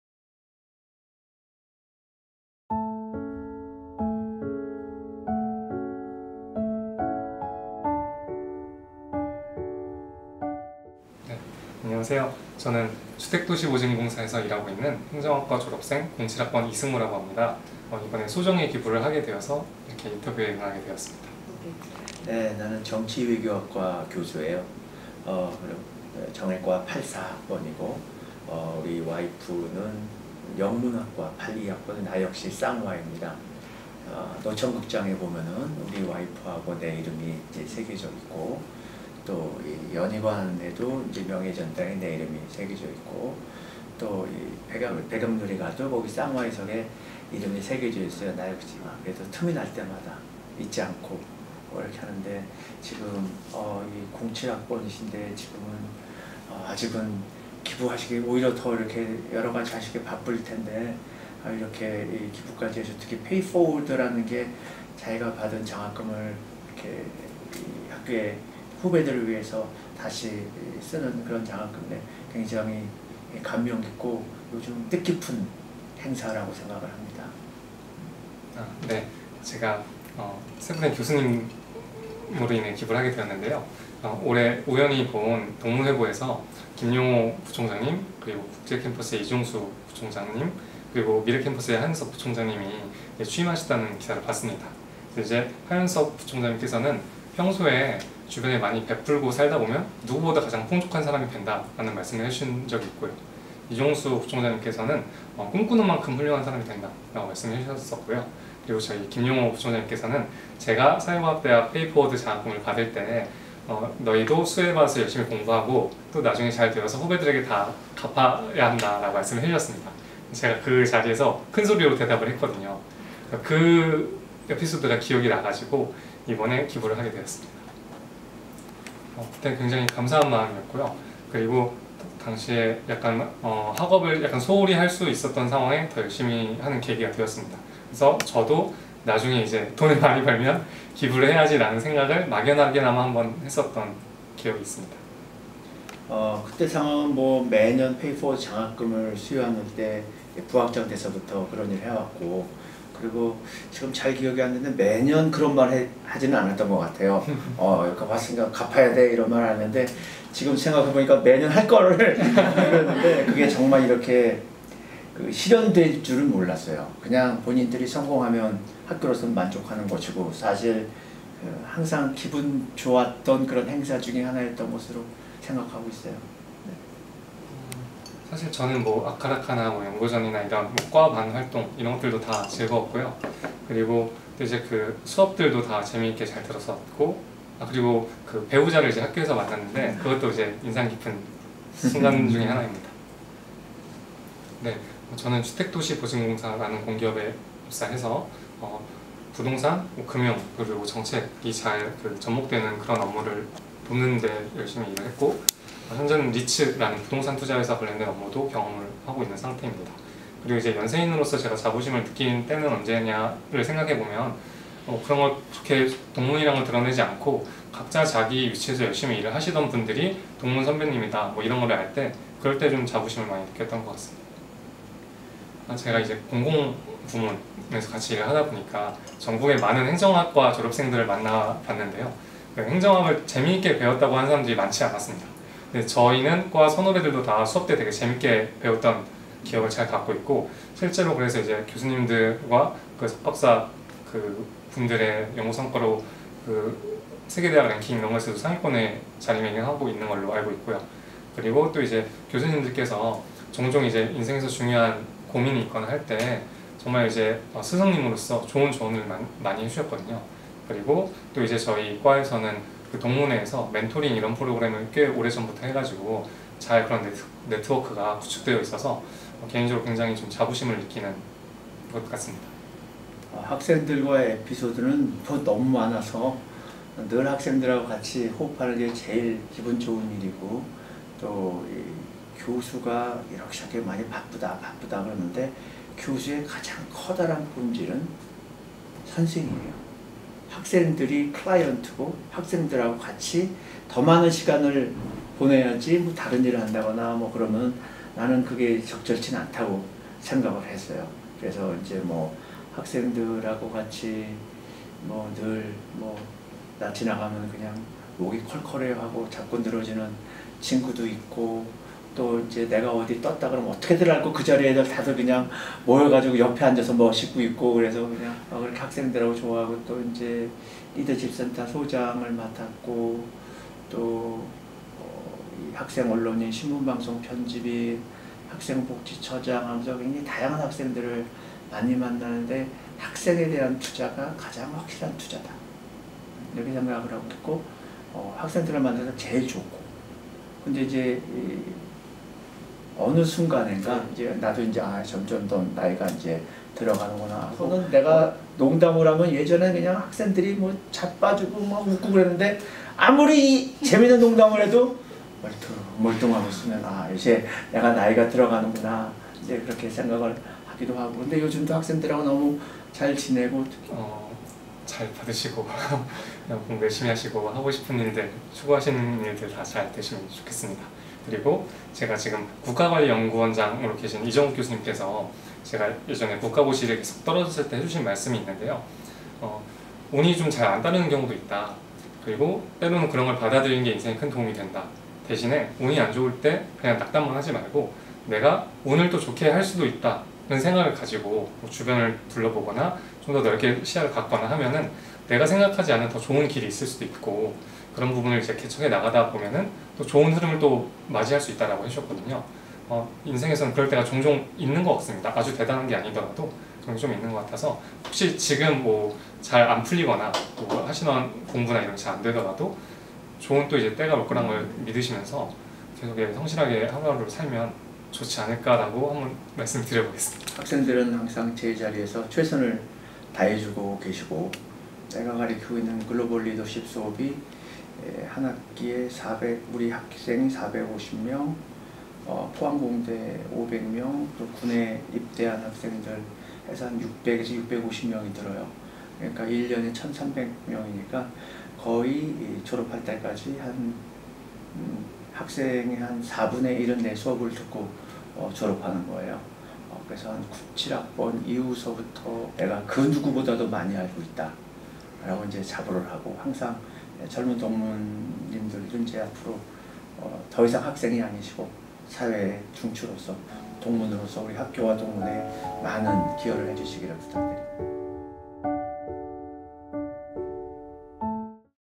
"나중에 돈을 많이 벌면 기부를 해야겠다고 생각했습니다." - Pay Forward 장학기금 기부자 이승우 동문님
안녕하세요, 연세대학교 학생 기부대사 더스피릿입니다.
지난 8월 23일, Pay Forward 장학기금 기부자 이승우 동문님과 김용호 행정·대외부총장님의 기부자 인터뷰가 있었습니다.
생생한 인터뷰 현장을 함께 만나보세요!!